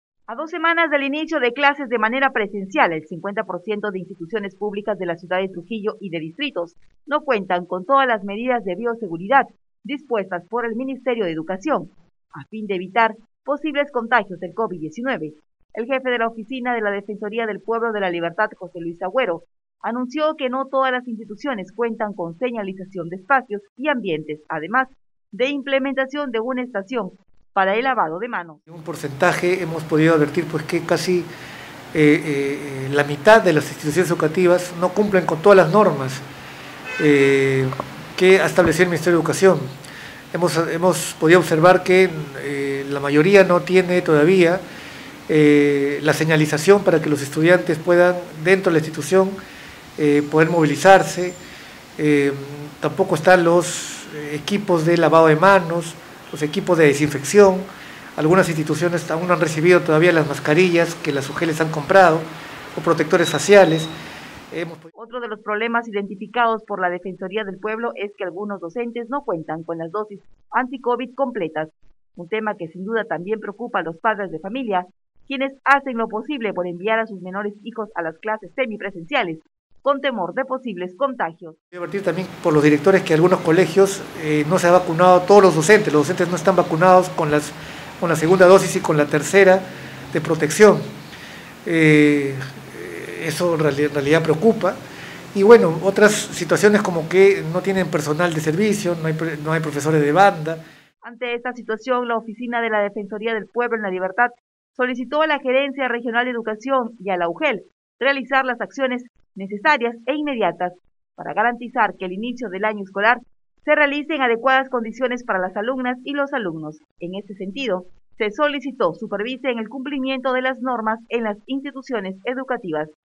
Defensoría del Pueblo: el 50% de colegios no cuentan con protocolos
El jefe de la oficina Defensorial de La Libertad, José Luis Agüero, señaló que, a dos semanas del inicio de clases de manera general en las instituciones públicas, aún existe un gran porcentaje de colegios que no cuentan con los protocolos de bioseguridad.

A dos semanas del inicio de clases de manera presencial, el 50 % de instituciones públicas de la ciudad de Trujillo y de distritos no cuentan con todas las medidas de bioseguridad dispuestas por el Ministerio de Educación. A fin de evitar posibles contagios del covid-19, el jefe de la oficina de la defensoría del pueblo de la Libertad, José Luis agüero, anunció que no todas las instituciones cuentan con señalización de espacios y ambientes además de implementación de una estación para el lavado de manos.

Otro de los problemas identificados por la Defensoría del Pueblo es que algunos docentes no cuentan con las dosis anticovid completas, un tema que sin duda también preocupa a los padres de familia quienes hacen lo posible por enviar a sus menores hijos a las clases semipresenciales, con temor de posibles contagios.

Ante esta situación, la oficina de la Defensoría del Pueblo en La Libertad solicitó a la Gerencia Regional de Educación y a las UGEL realizar las acciones necesarias e inmediatas para garantizar que el inicio del año escolar se realice en adecuadas condiciones para las alumnas y los alumnos. En ese sentido, se solicitó supervisen el cumplimiento de las normas en las instituciones educativas.